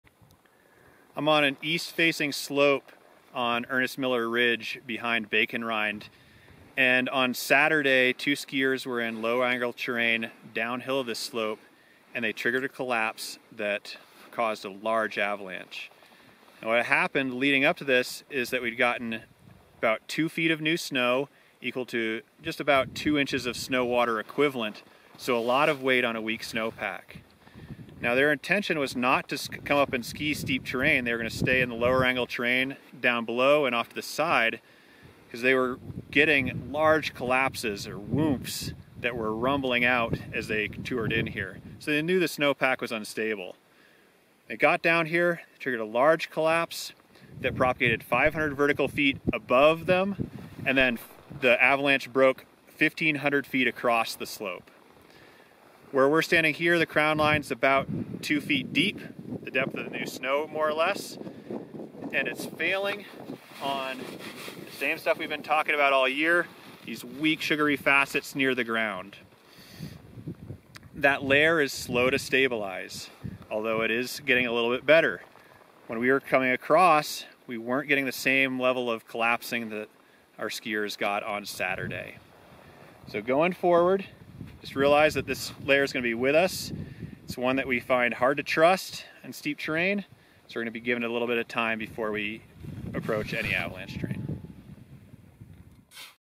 1500 Wide, Human Triggered Avalanche - 1 February 2021
We toured into Ernie Miller Ridge to check out a large, human-triggered avalanche. The skiers triggered the slide from out of the way on the south side of the avalanche. The slide then propagated 500 vertical feet uphill and 1500 feet across. It failed on weak depth hoar near the ground.

The sensitivity has decreased since Saturday. While natural avalanches are unlikely, human-triggered slides remain a scary possibility.